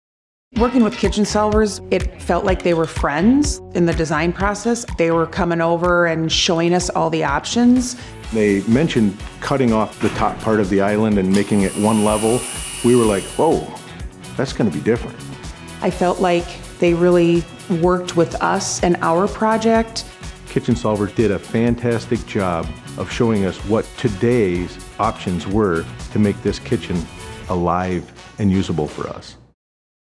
Kitchen Upgrade on your 2023 To-Do List?
Are you looking to update your kitchen, but not sure where to start or what your options are? Kitchen Solvers uses a consultative approach to determine what your functionality and style needs are and delivers a kitchen you may not have even been able to imagine.